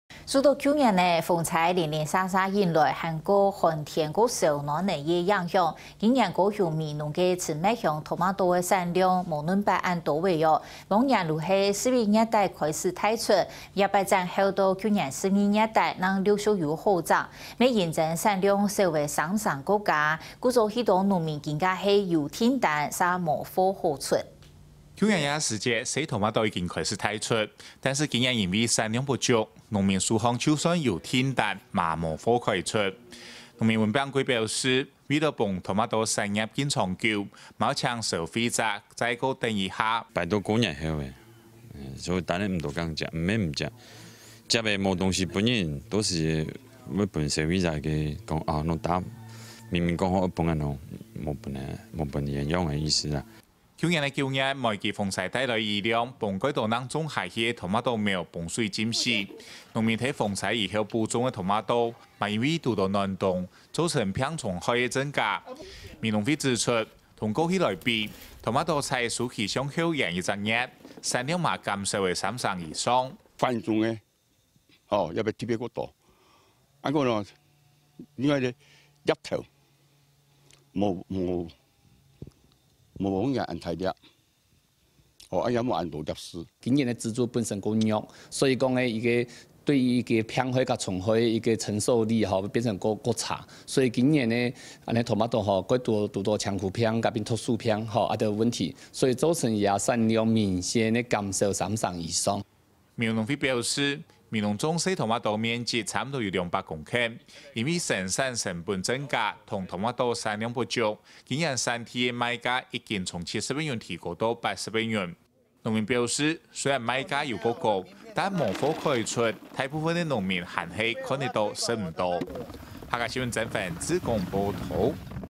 橙蜜香番茄減產3成 農民被訂單追著跑【客家新聞20170106】
受到去年連續颱風及暖冬氣候的影響，今年高雄美濃的橙蜜香番茄產量不如以往，往年11月底開始的產季，也延到12月底才陸續採收，也因為產量減少3成以上，農民現在出現，被訂單追著跑的窘況。